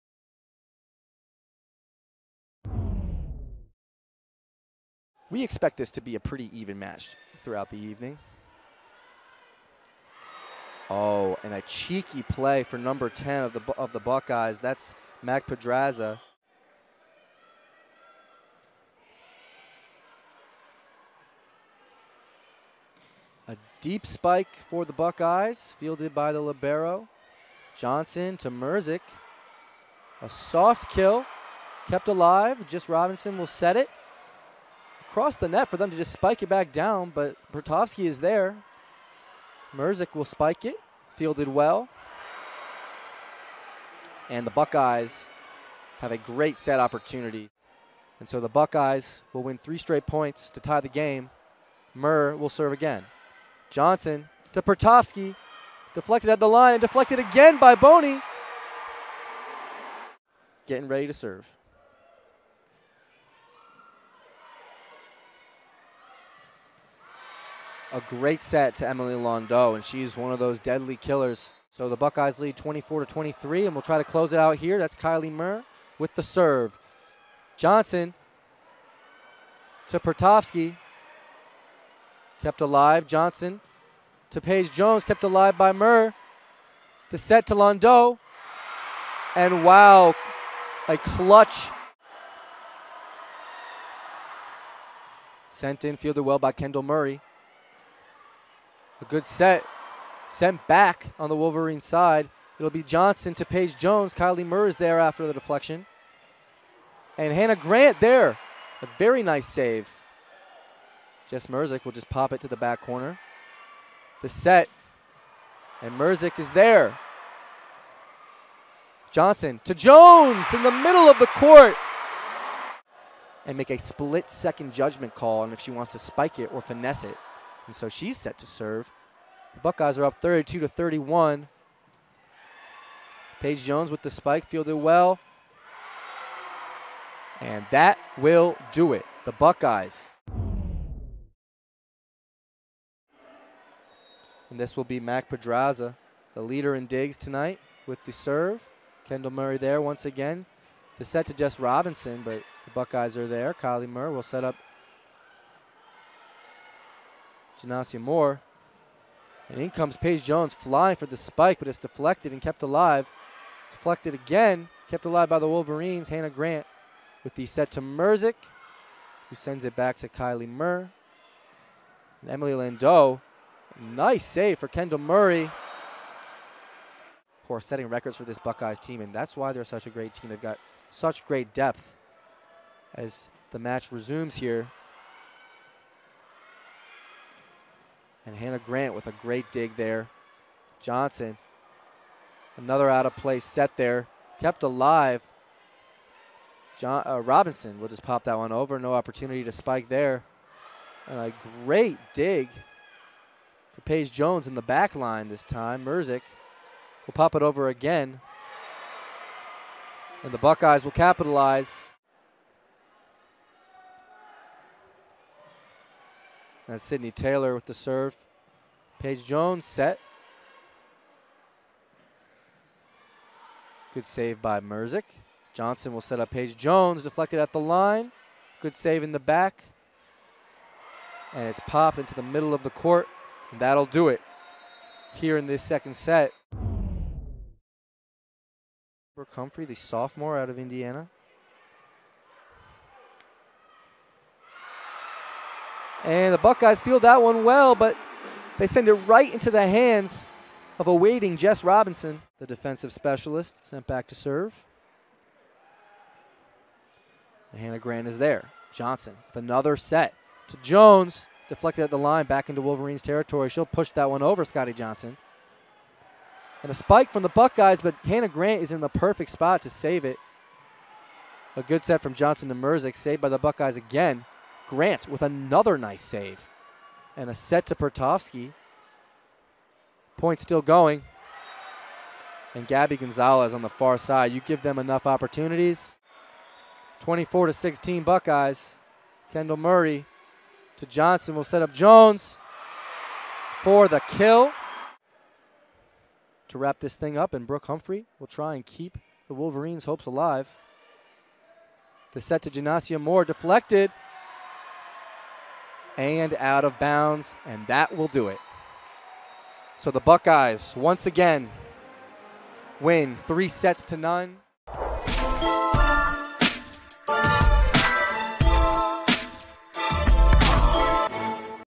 Ohio State at Michigan | Big Ten Volleyball | Highlights | Oct. 29, 2021
Ohio State goes on the road and gets the win against Ohio State.

Watch live games and replays from Big Ten sports with BTN +